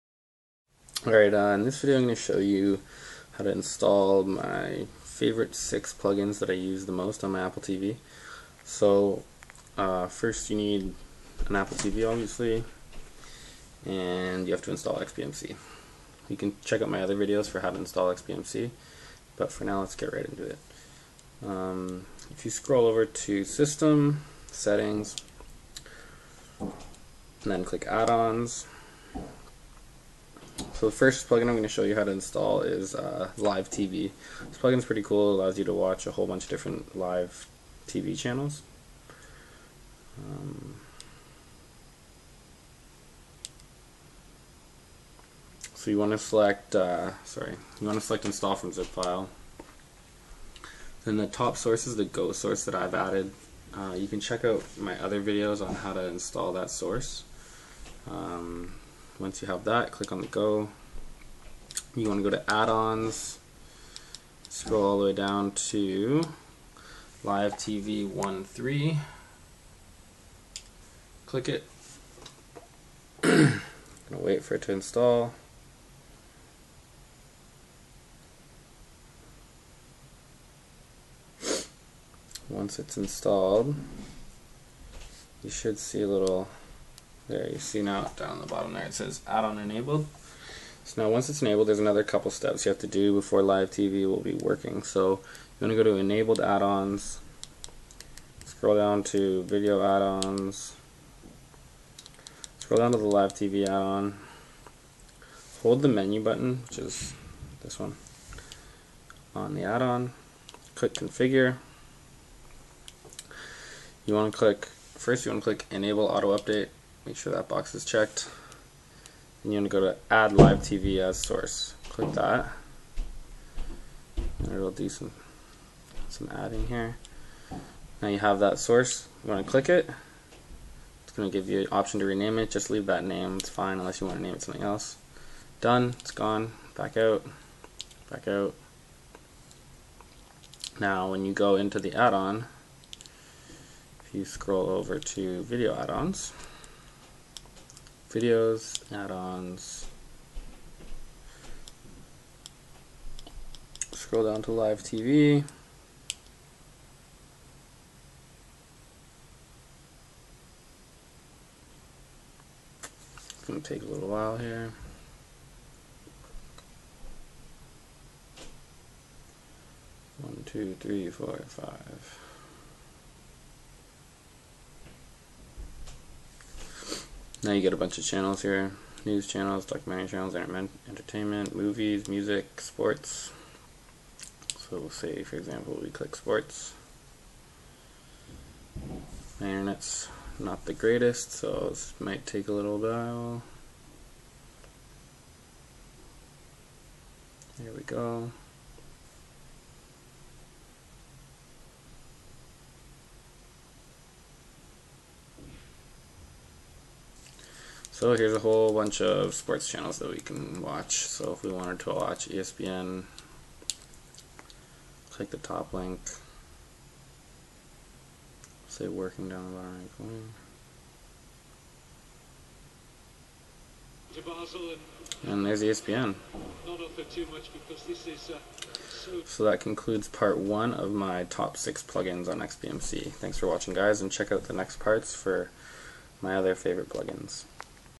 How to watch Live TV on your apple tv using LiveTV on XBMC
Part 1 of my Top Plugins Series:
This video shows you how to install and use the LiveTV plugin on your apple tv on XBMC.

To Jailbreak your appleTV and install XBMC: (new updated video coming soon)